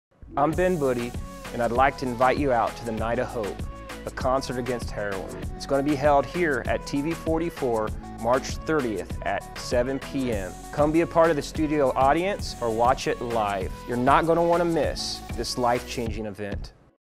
Night of Hope, Live at TV-44
You're Invited to the Night of Hope, March 30, 7pm, at TV-44. Join us for this live, in studio broadcast.